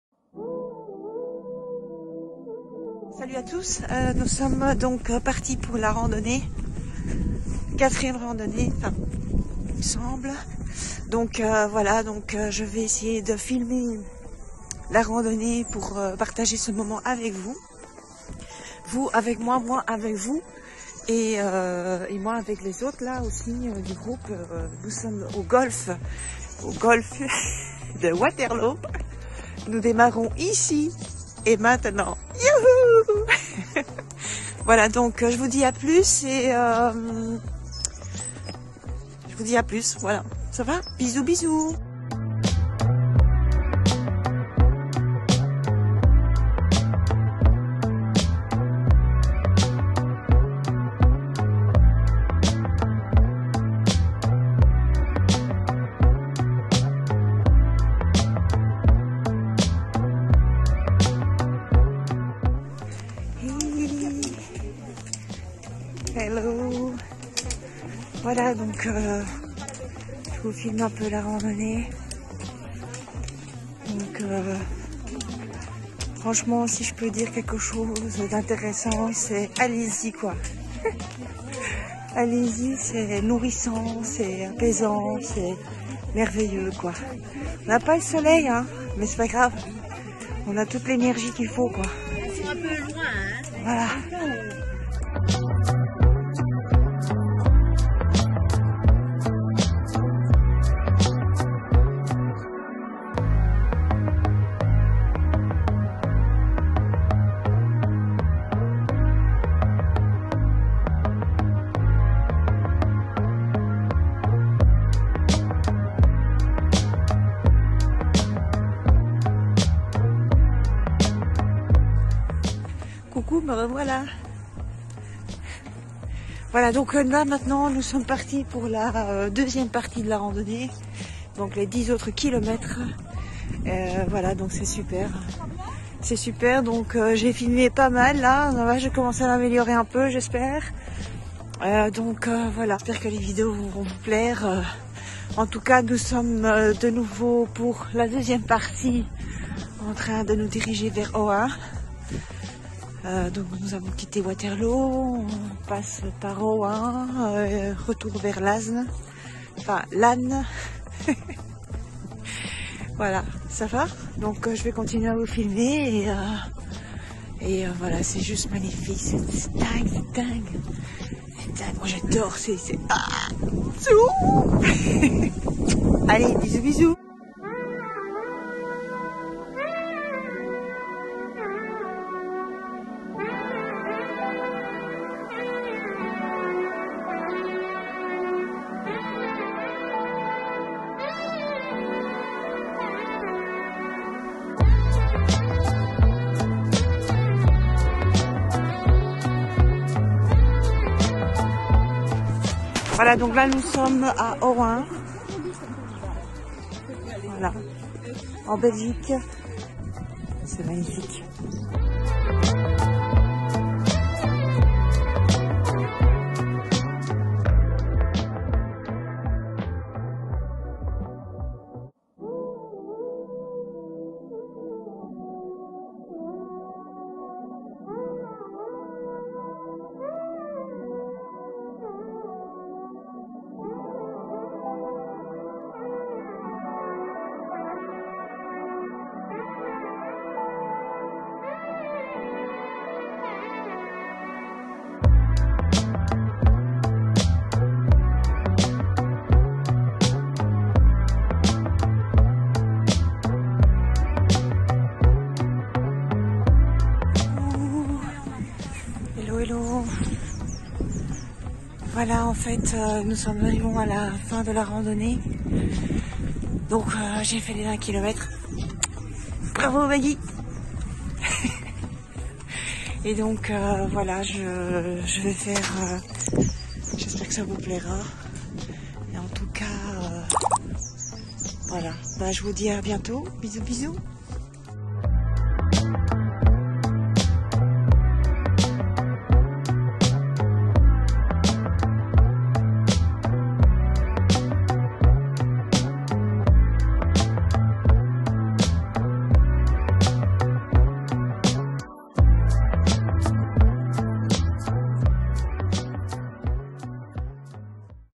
Petite randonnée en Belgique/Ohain🍀🌼🍀
Hello, bonjour à tous ! J'ai commencé à faire des randonnées en Belgique et j'ai décidé de poster mes vidéos sur YouTube pour vous partager mes aventures avec la nature.
Je suis hyper contente de vivre ces moments magiques avec vous.
Aujourd'hui, je vous emmène avec moi à Ohain pour une petite randonnée.
J'espère que ça vous plaira, bisou, bisou!